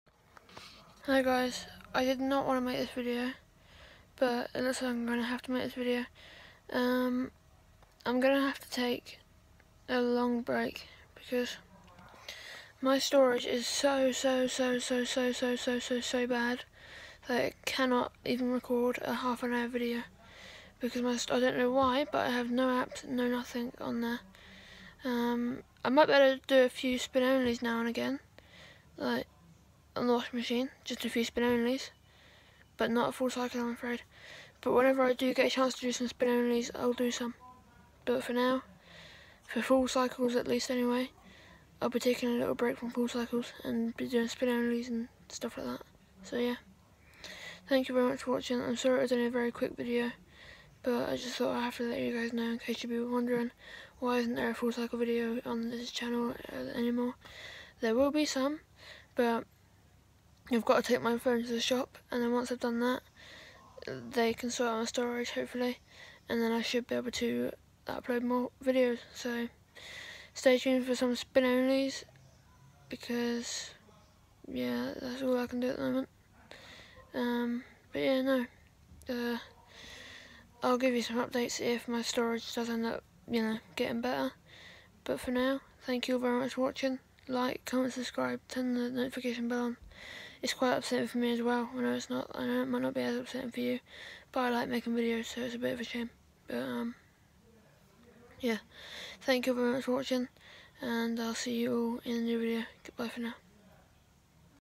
Taking a small break from full cycle videos due to storage (please read description)
Like comment subscribe and turn on the notification bell please and like I said in the video my storage is seriously the worst it has ever been to the welcome back Full circle video of the candy will be delayed but I’ll keep you posted on what goes on with my storage